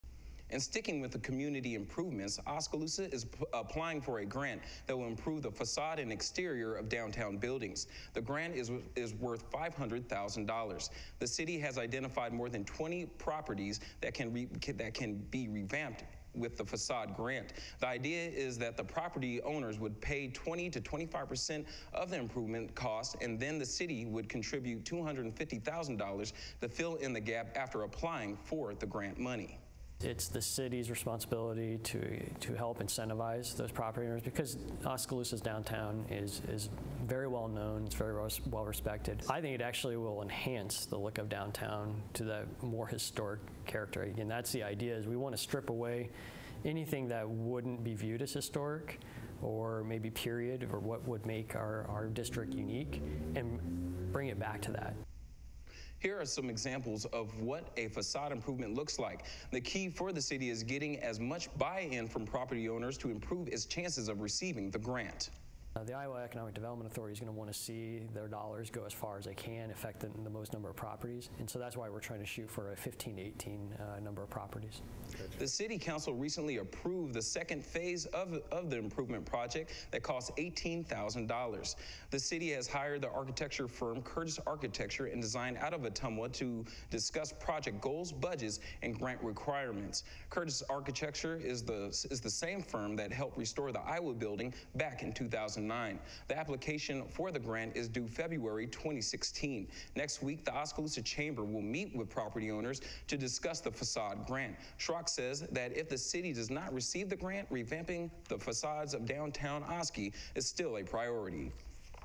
City Seeking Facade Grant for Downtown Osky
Oskaloosa is applying for a grant that will improve the facade and exterior of downtown buildings. The grant is worth $500,000. The city has identified more than 20 properties that can revamp its facade.